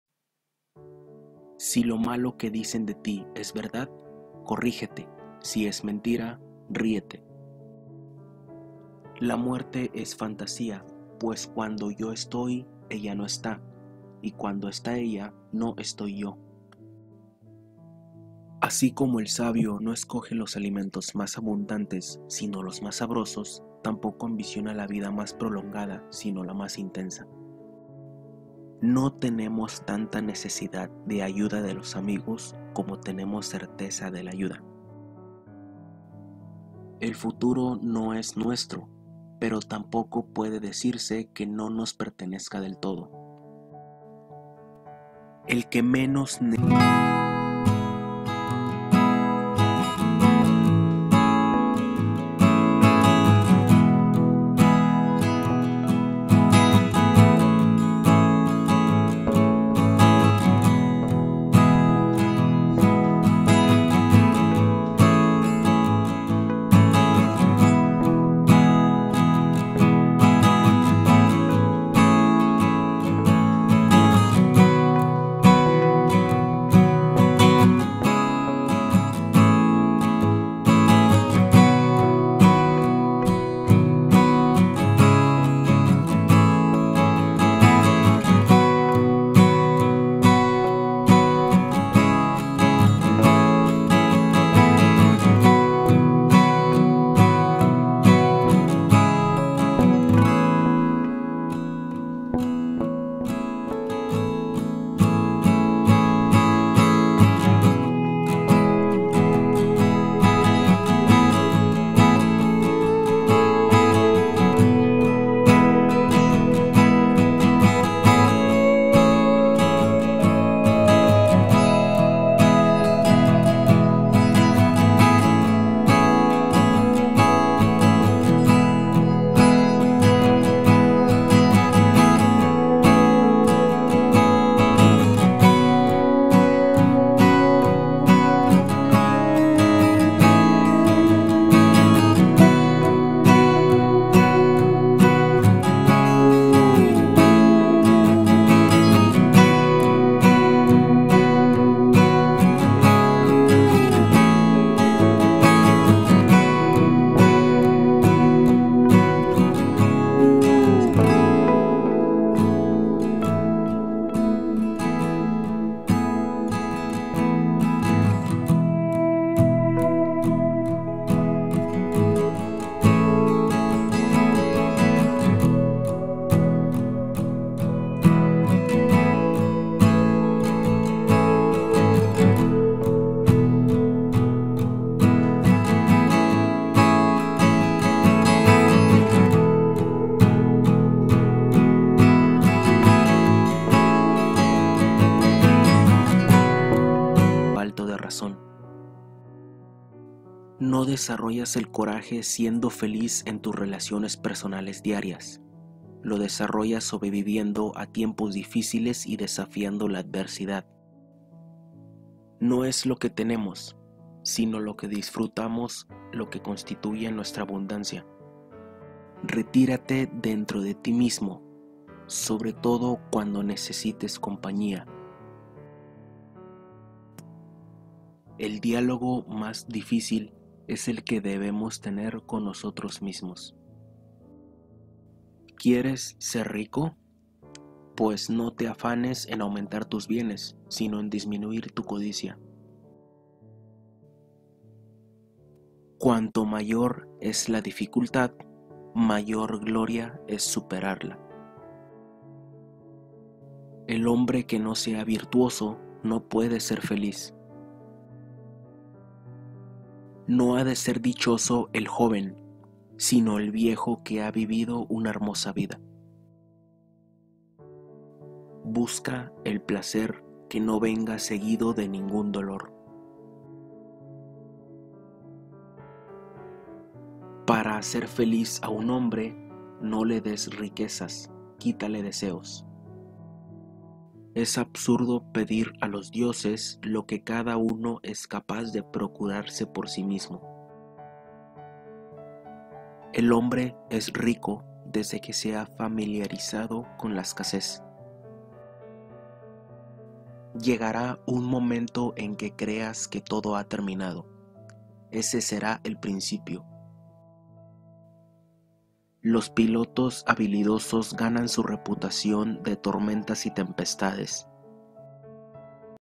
▶ Las FRASES y CITAS de EPICURO mas SORPRENDENTES y SABIAS del toda la epoca. ⭐
➡Sorpréndete con las frases y citas de Epicuro mas sorprendentes de toda la época, úsalas para motivarte y volverte una MEJOR PERSONA⭐